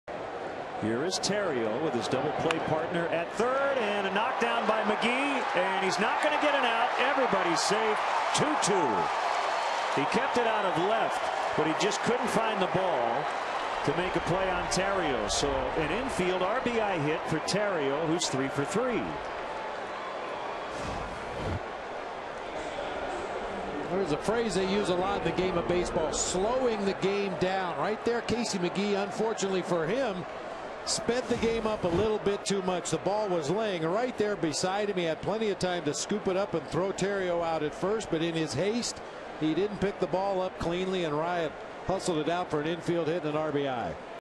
2010/06/10 Theriot's RBI single
CHC@MIL: Theriot plates Castro with an infield single